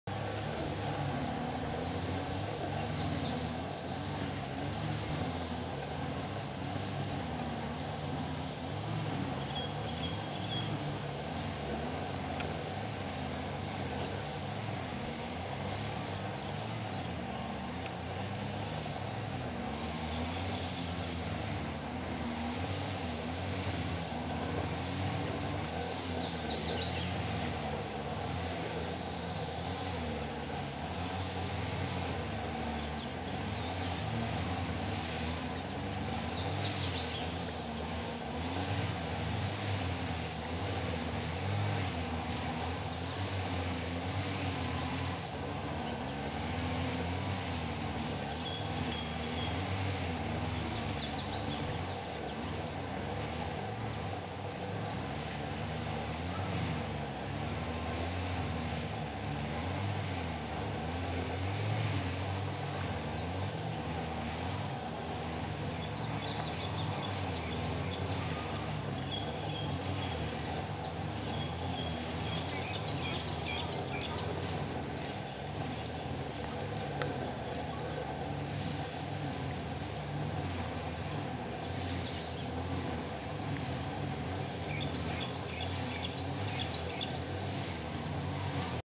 Galapagos tortoise eating
On Isla Santa Cruz, Galapagos.  The buzzing sound in the background is a line trimmer -- this tortoise is wild but it's on someone's private land here and that land owner is trimming the vegetation this day.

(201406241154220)